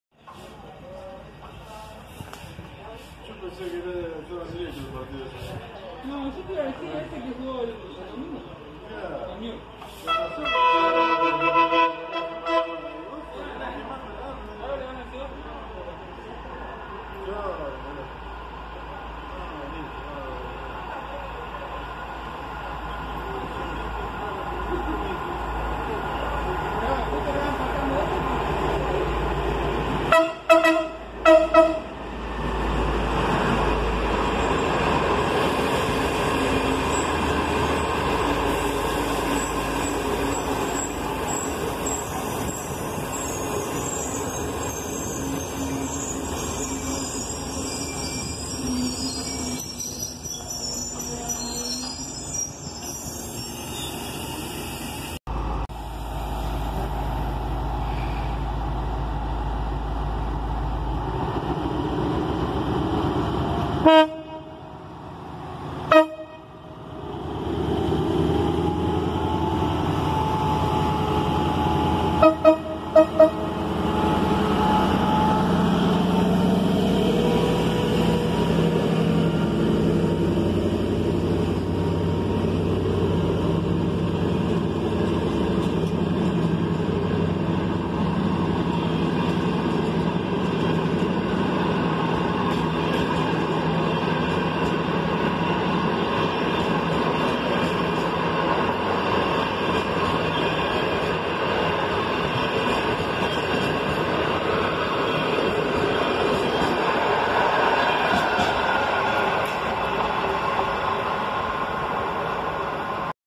CNR Dalian CKD8G 0011 con el Tren N' 277 Llegando a San Nicolás (0070) 2-12-2020
CNR Dalian CKD8G 0011 con el Tren N' 277 Llegando a San Nicolás
CNR Dalian CKD8G 0011 with train number 277 arriving at San Nicolás